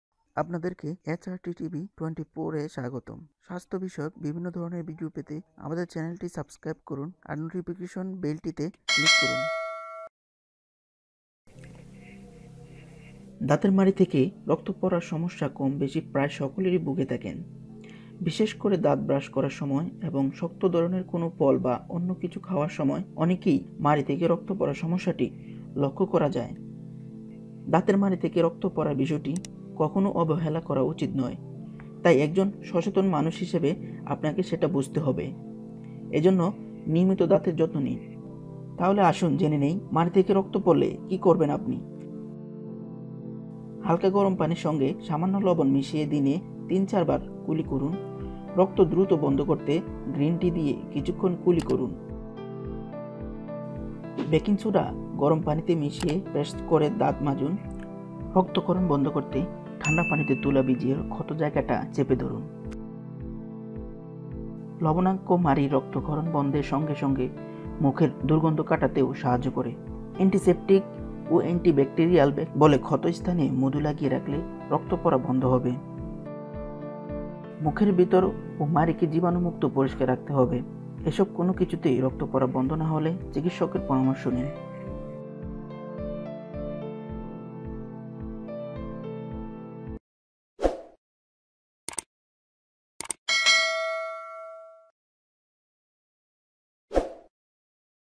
দাঁতের মাড়ির রক্ত পড়া বন্ধ করুন ঘরোয়া ৪ উপায়ে_দাঁতের মাড়ি থেকে রক্ত পড়লে করণীয়
আসুন জেনে নিন মাড়ি থেকে রক্ত পড়লে কী করবেন.

* হালকা গরম পানির সঙ্গে সামান্য লবণ মিশিয়ে দিনে তিন-চার বার কুলি করুন
* রক্ত দ্রুত বন্ধ করতে গ্রিন টি দিয়ে কিছুক্ষণ কুলি করুন
* বেকিং সোডা গরম পানিতে মিশিয়ে পেস্ট করে দাঁত মাজুন
* রক্তক্ষরণ বন্ধ করতে ঠাণ্ডা পানিতে তুলা ভিজিয়ে ক্ষত জায়গাটায় চেপে ধরুন
* লবঙ্গ মাড়ির রক্তক্ষরণ বন্ধের সঙ্গে সঙ্গে মুখের দুর্গন্ধ কাটাতেও সাহায্য করে
* অ্যান্টিসেপ্টিক ও অ্যান্টিব্যাকটেরিয়াল বলে ক্ষত স্থানে মধু লাগিয়ে রাখলে রক্ত পড়া বন্ধ হবে
* মুখের ভেতর ও মাড়িকে জীবাণুমুক্ত-পরিষ্কার রাখতে হবে।

এসব কোনও কিছুতেই রক্ত পড়া বন্ধ না হলে চিকিৎসকের পরামর্শ নিন।